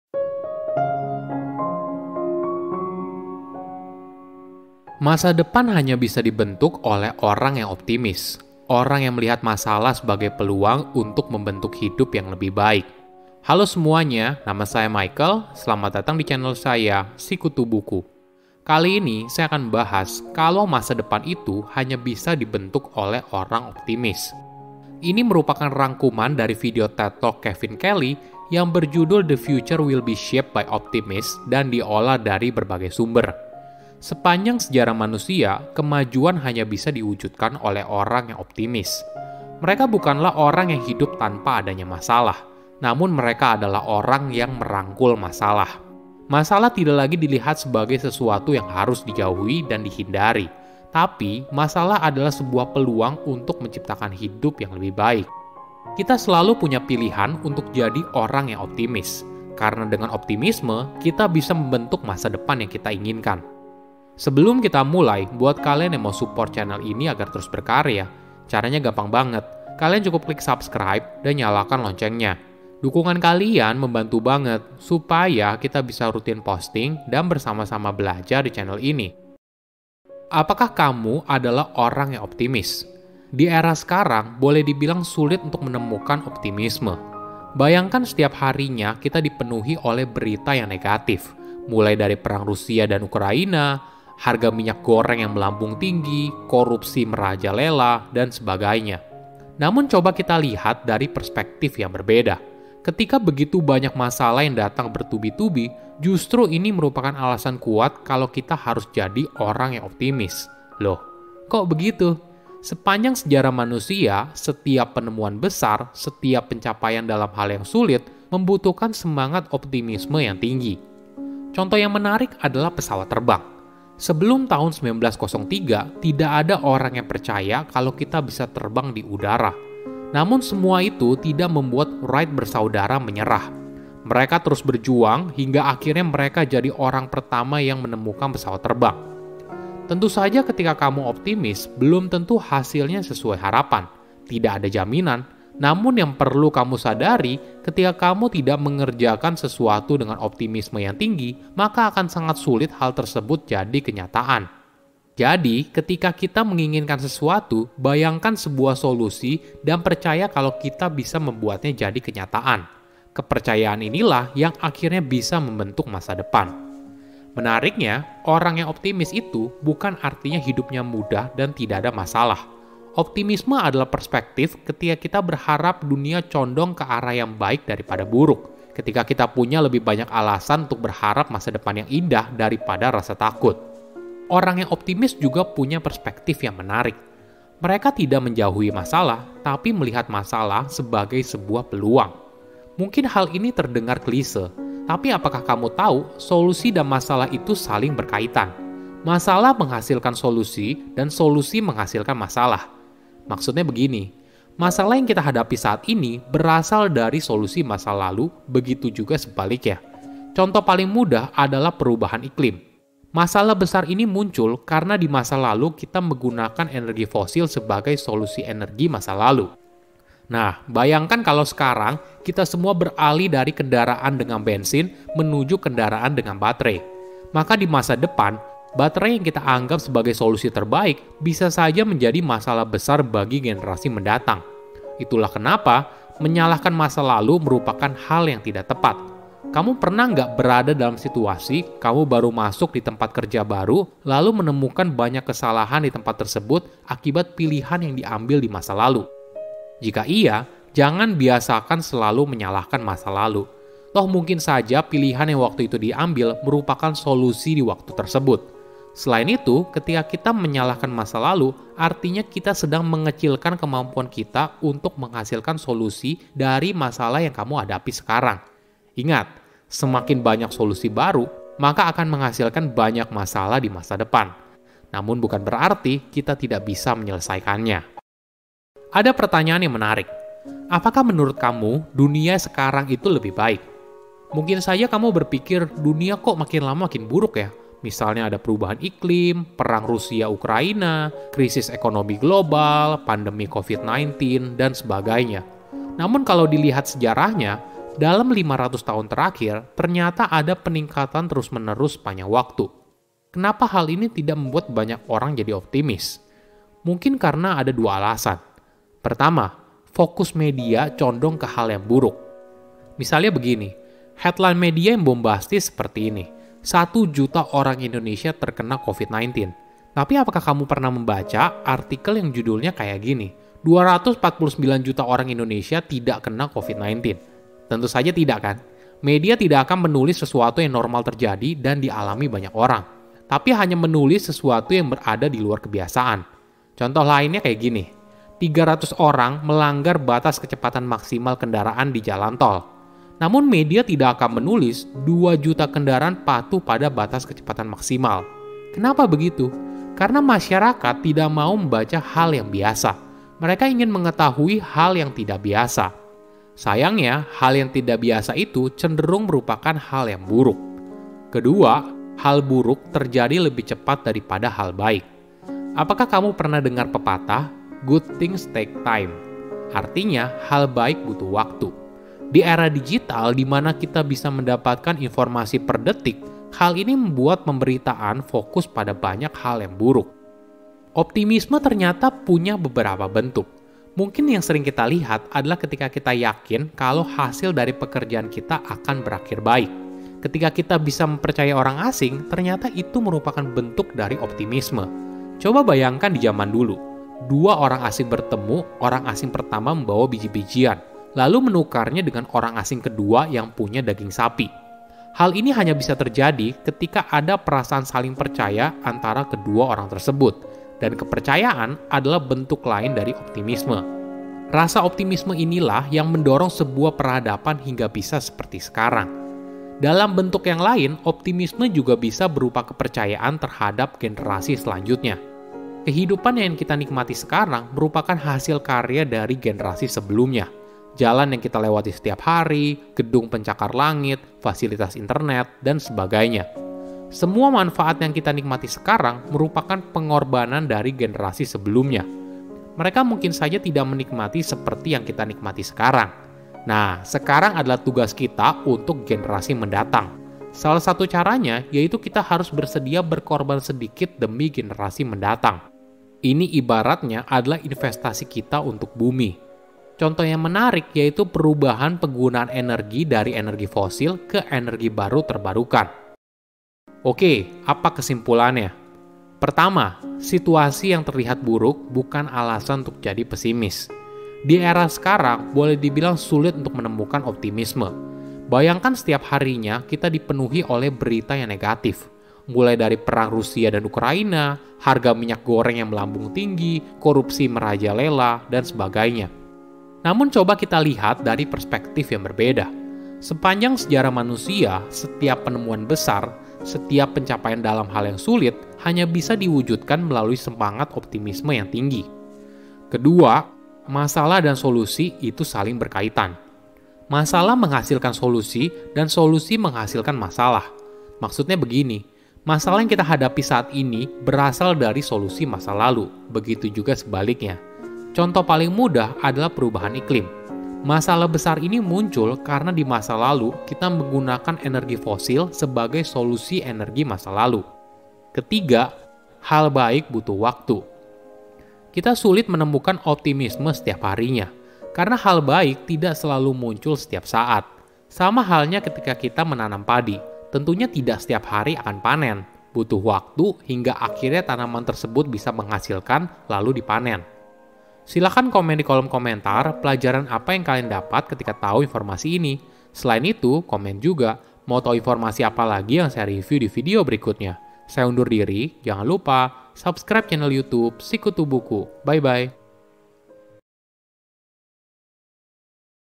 Masa Depan Hanya Bisa Dibentuk oleh Orang Optimis
Saya membahas kalau masa depan itu hanya bisa dibentuk oleh orang optimis. Ini merupakan rangkuman dari video TED Talk Kevin Kelly yang berjudul The Future will be Shaped by Optimists dan diolah dari berbagai sumber. Sepanjang sejarah manusia, kemajuan hanya bisa diwujudkan oleh orang yang optimis. Mereka bukanlah orang yang hidup tanpa adanya masalah, namun mereka adalah orang yang merangkul masalah. Masalah tidak lagi dilihat sebagai sesuatu yang dijauhi dan dihindari, tapi masalah adalah sebuah peluang untuk menciptakan hidup yang lebih baik. Kita selalu punya pilihan untuk jadi orang yang optimis, karena dengan optimisme, kita bisa membentuk masa depan yang kita inginkan.

Sekarang, Podcast Si Kutu Buku sudah bergabung dengan Podcast Network Asia dan Podmetrics!